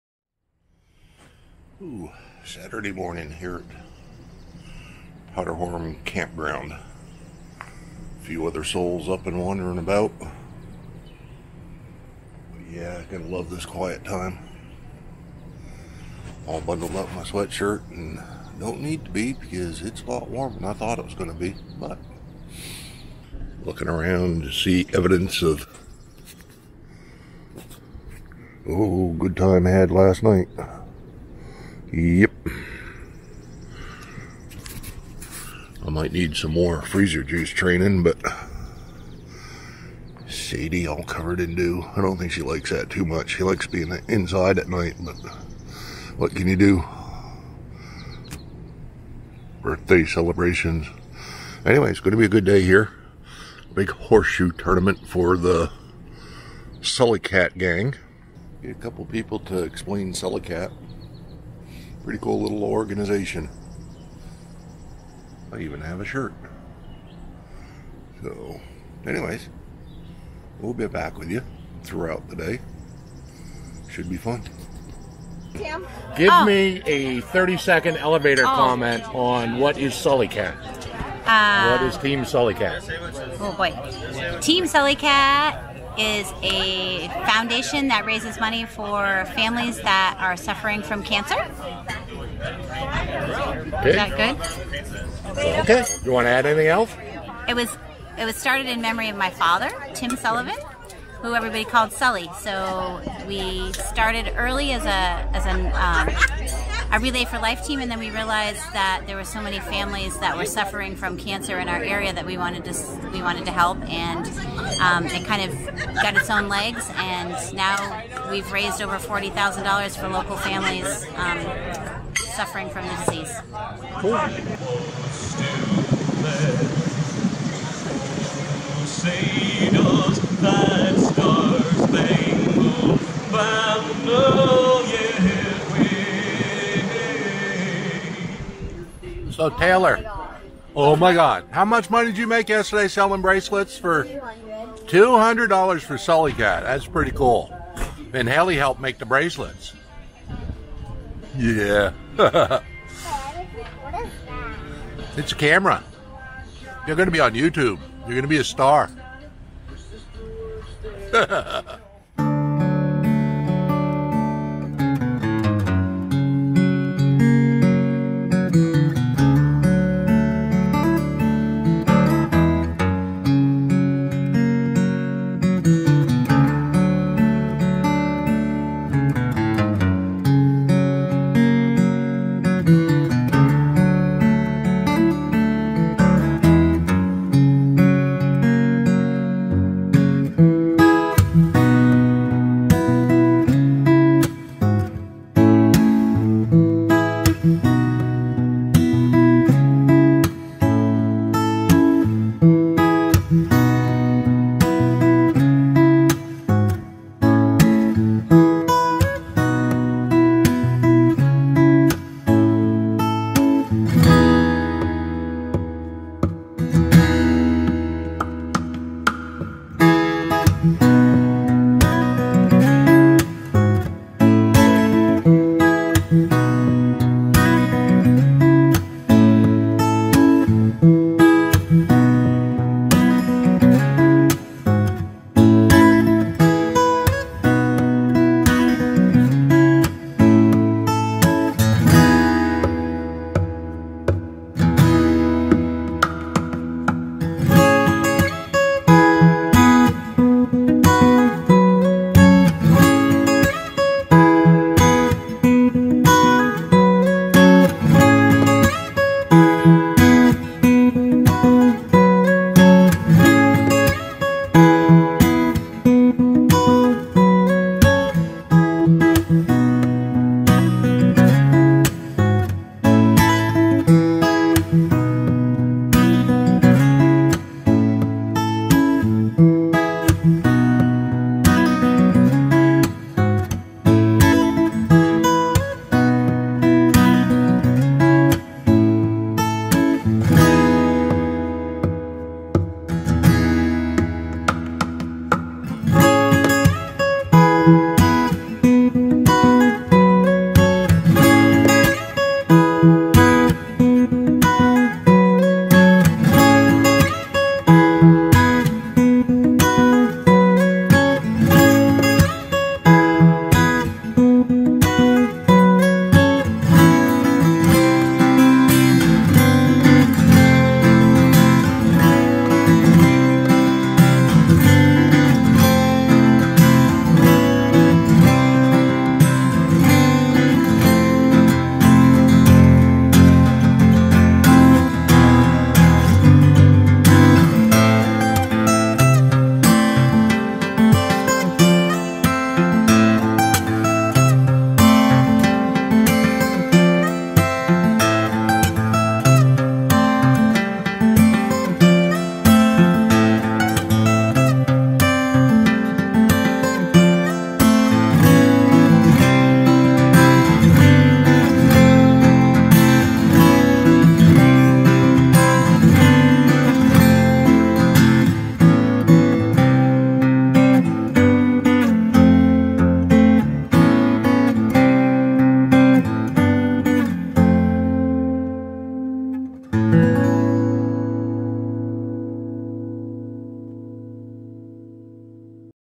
101A OOB Camping and Team Sullycat
Highlights from Old Orchard Beach Camping and Team Sullycat.  So much fun with great people.